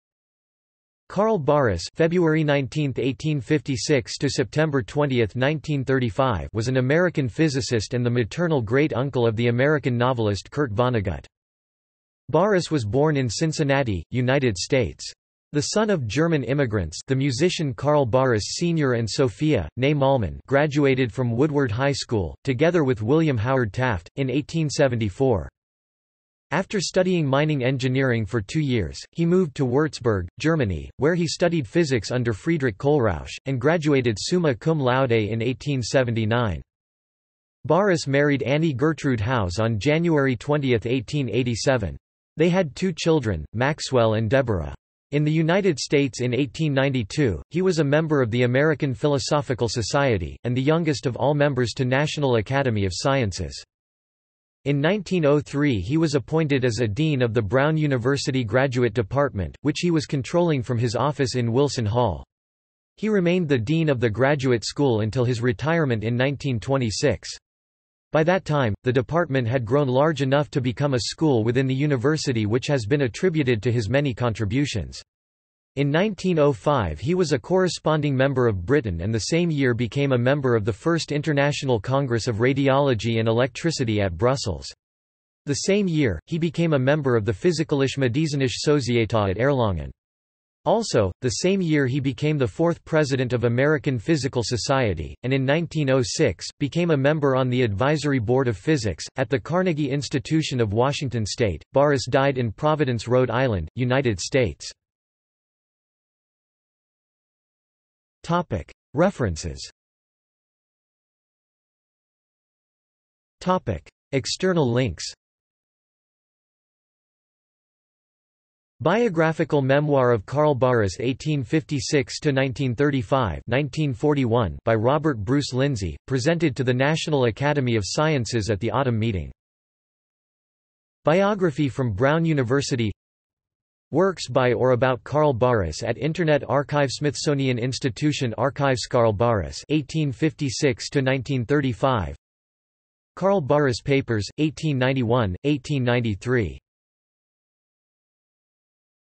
Carl Barus | Wikipedia audio article
This is an audio version of the Wikipedia Article:
Carl Barus

- Socrates

SUMMARY
Carl Barus (February 19, 1856 – September 20, 1935) was an American physicist and the maternal great-uncle of the American novelist Kurt Vonnegut.
Barus was born in Cincinnati, United States. The son of German immigrants (the musician Carl Barus, Sr. and Sophia, nee Möllmann) graduated from Woodward High School, together with William Howard Taft, in 1874.
After studying mining engineering for two years, he moved to Würzburg, Germany, where he studied physics under Friedrich Kohlrausch, and graduated summa cum laude in 1879.
Barus married Annie Gertrude Howes on January 20, 1887. They had two children, Maxwell and Deborah. In the United States in 1892, he was a member of the American Philosophical Society, and the youngest of all members to National Academy of Sciences.
In 1903 he was appointed as a dean of the Brown University Graduate Department, which he was controlling from his office in Wilson Hall. He remained the dean of the graduate school until his retirement in 1926. By that time, the department had grown large enough to become a school within the university which has been attributed to his many contributions. In 1905 he was a corresponding member of Britain and the same year became a member of the First International Congress of Radiology and Electricity at Brussels. The same year, he became a member of the Physikalisch-Medizinische Sozietät at Erlangen. Also, the same year he became the fourth president of American Physical Society, and in 1906, became a member on the advisory board of physics, at the Carnegie Institution of Washington state.Barus died in Providence, Rhode Island, United States.